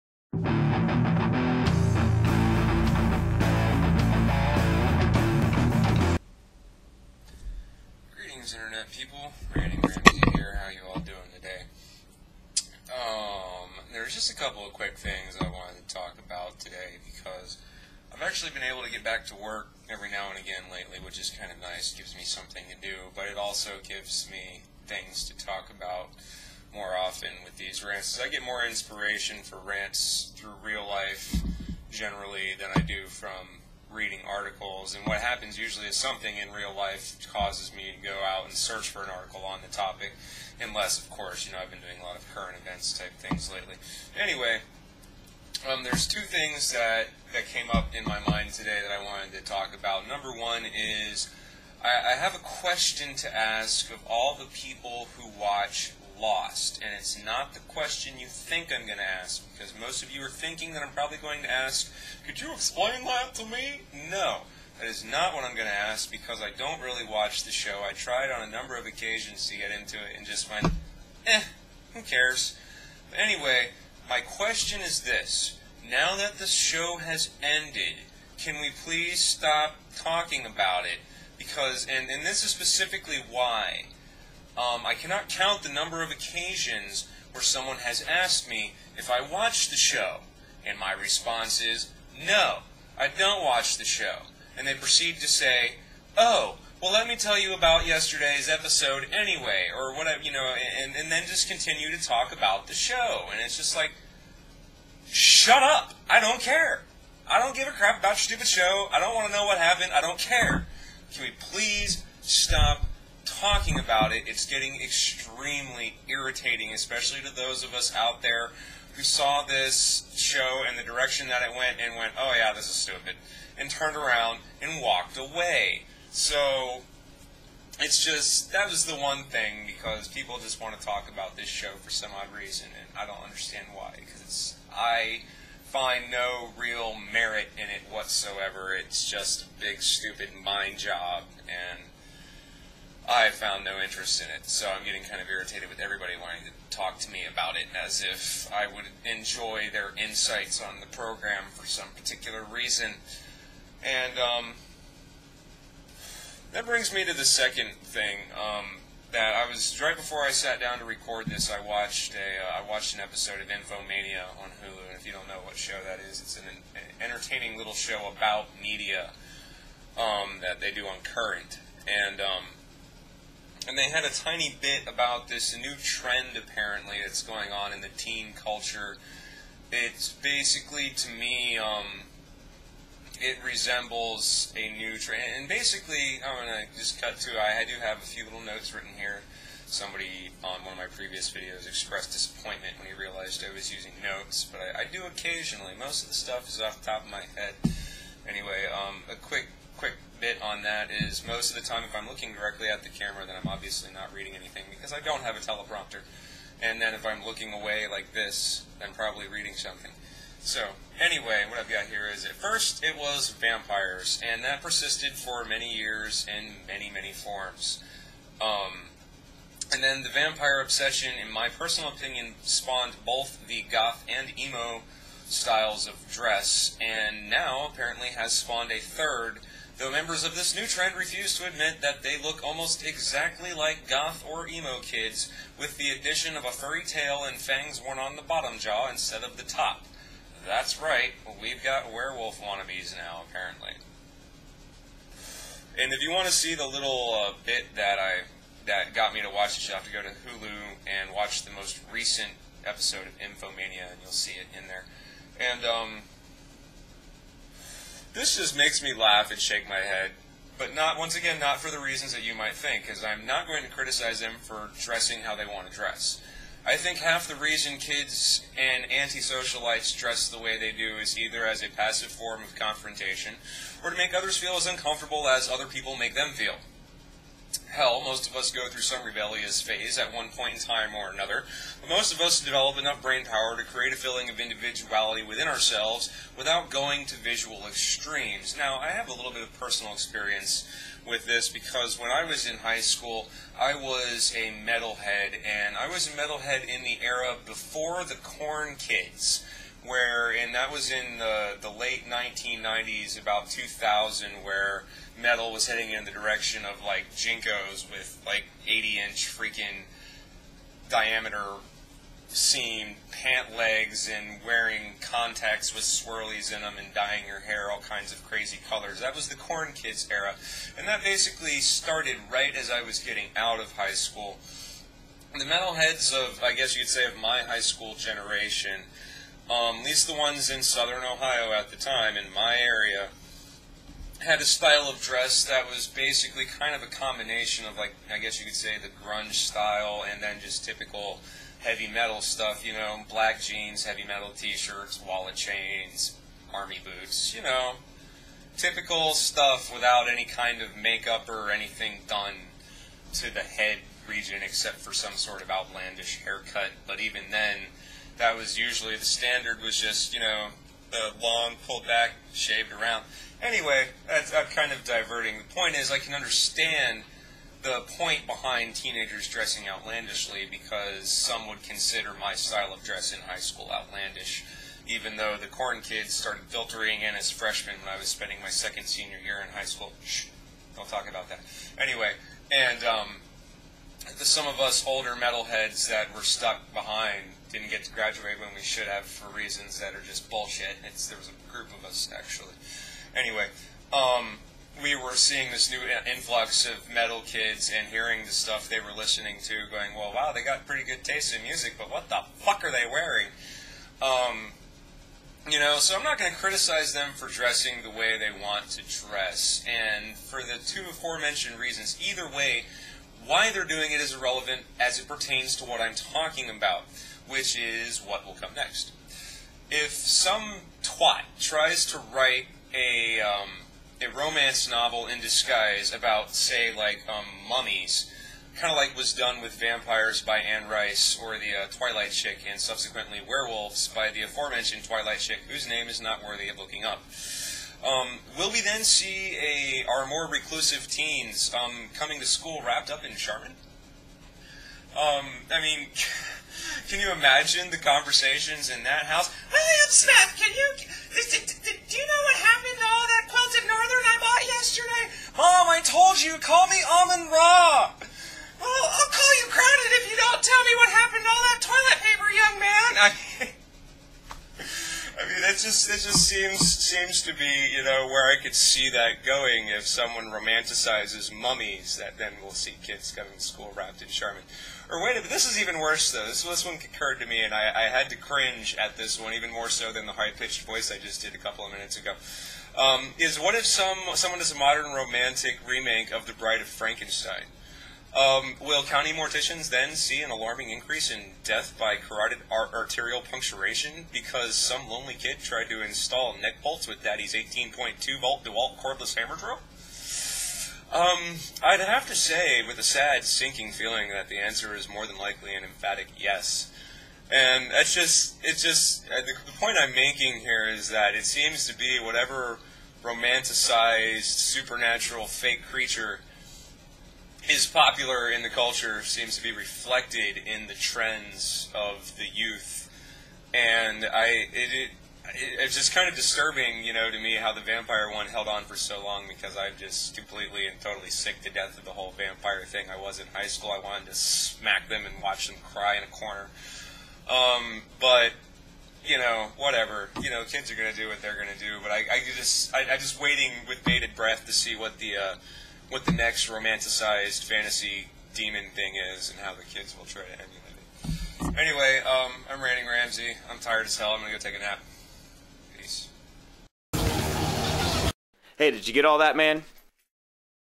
Can We Stop Talking About "Lost" Now? / Werewolf-Wannabe Teenagers
Seriously, people who liked "Lost" quit trying to talk about it with those of us that didn't like it!    Also, these kids and their crazy trends...